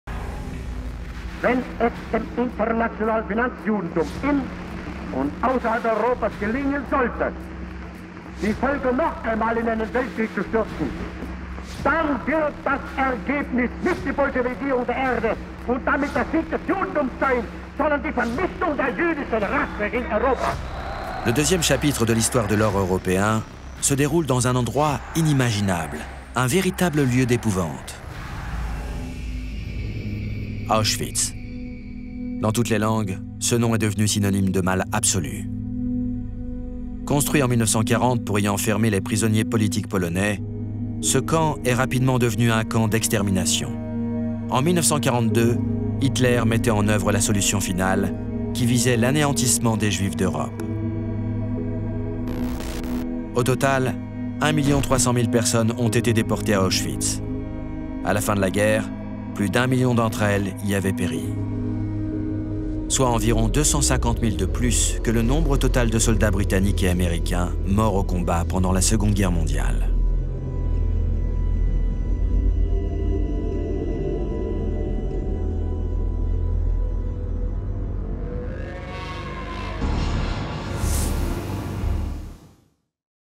Auschwitz : le camp de l'horreur nazie
Construit en 1940 pour y enfermer les prisonniers politiques polonais, ce camp est rapidement devenu un camp d'extermination.